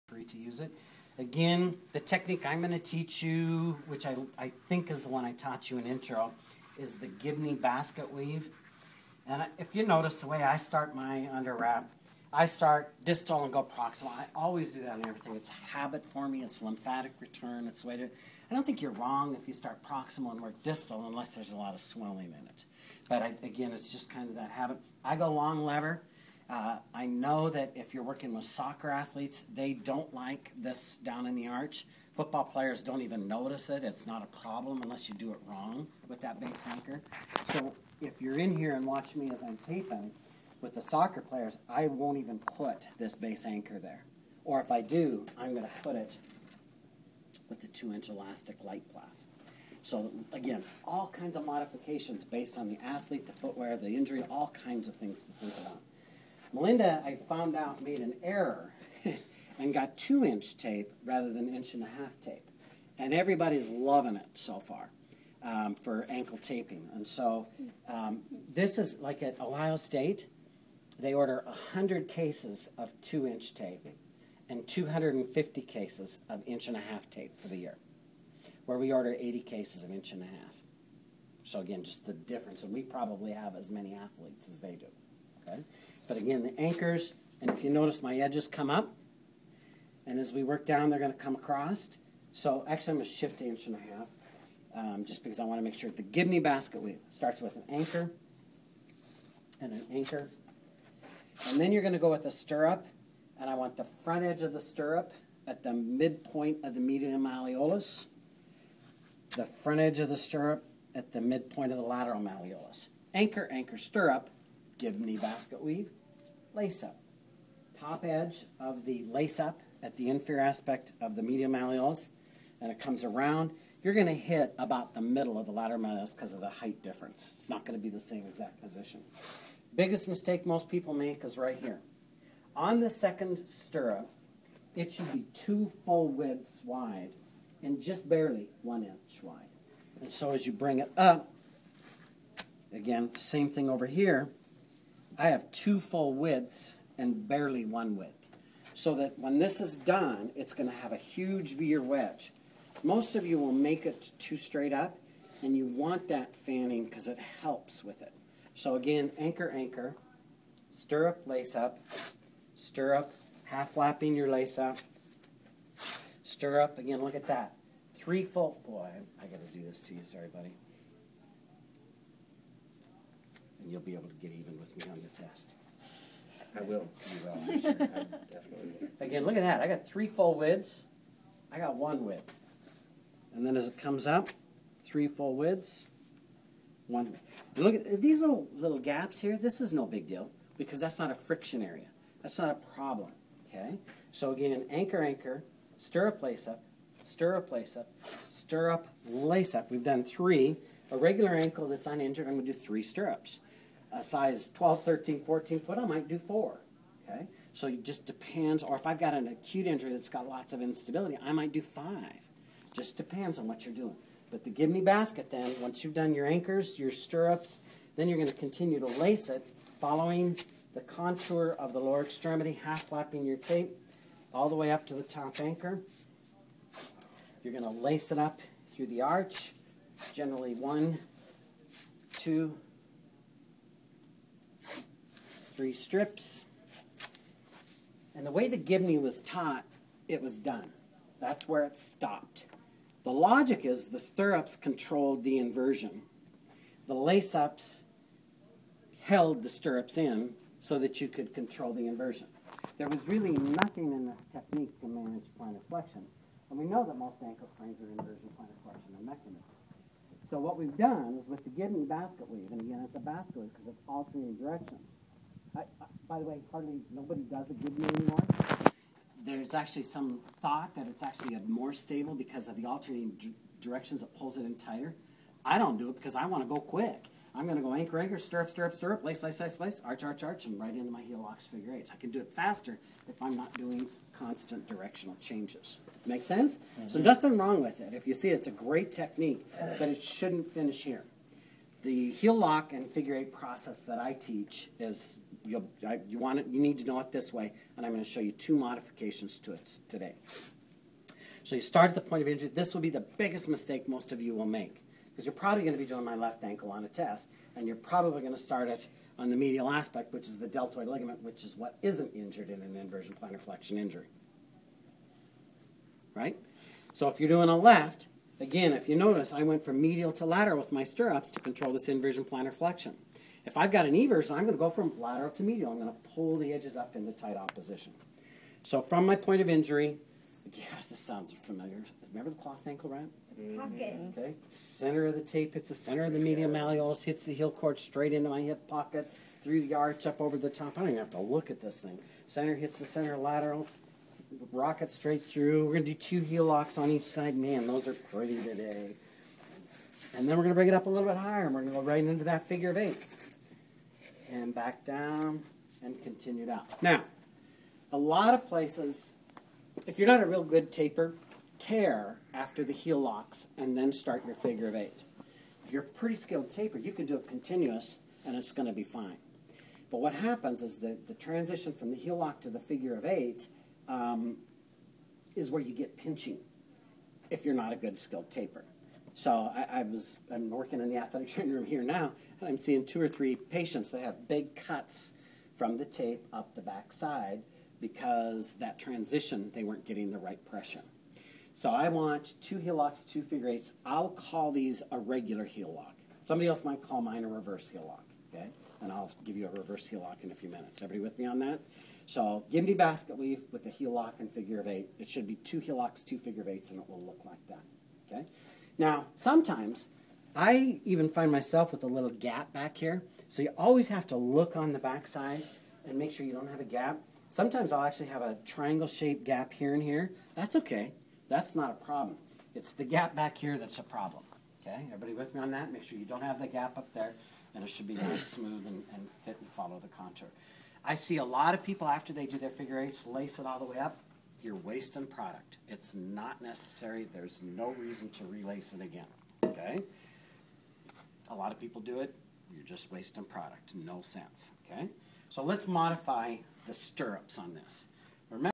Gibney Basket Weave - Ankle Taping
This is the Gibney Basket Weave for ankle injuries. Taught by Russ Richardson EdD, ATC for "Lower Extremities in Athletic Training."